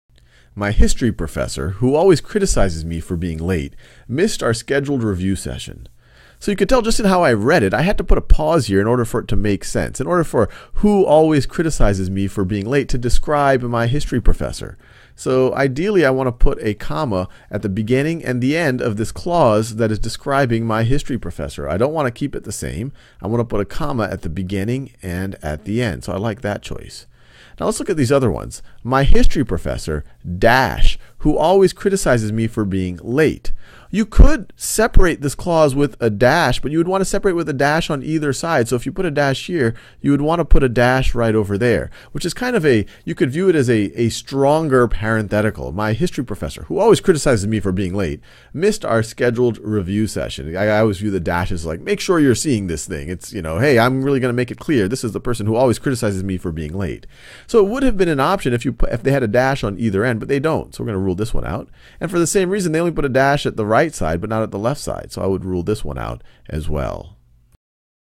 Writing: Parentheticals — Basic example | Writing & Language | SAT | Khan Academy
Watch Sal work through a basic nonrestrictive and parenthetical elements question from the SAT Writing and Language Test.